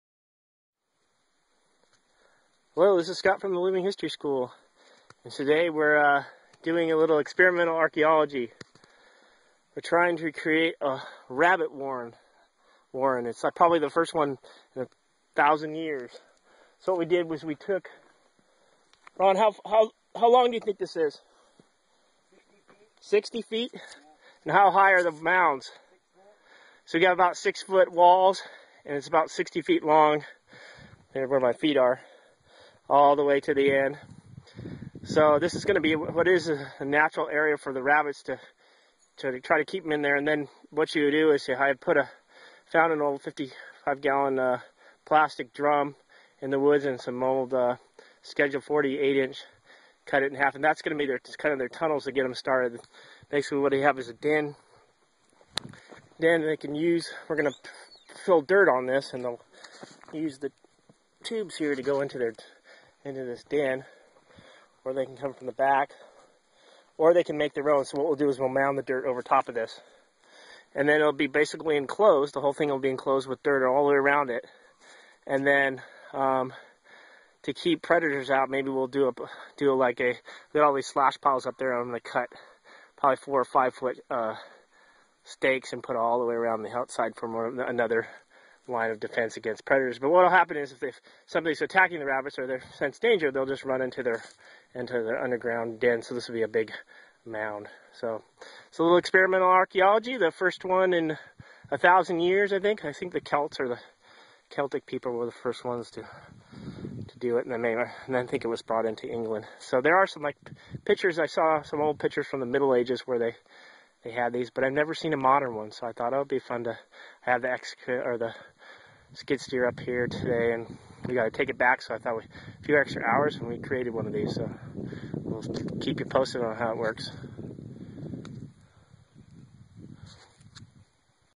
Building a Rabbit Warren Pillow Mound. A Test in Experimental Archaeology
A rabbit warren pillow mound is an enclosed establishment of animal husbandry dedicated to the raising of rabbits for meat and fur also called a Cony-Garth Here is a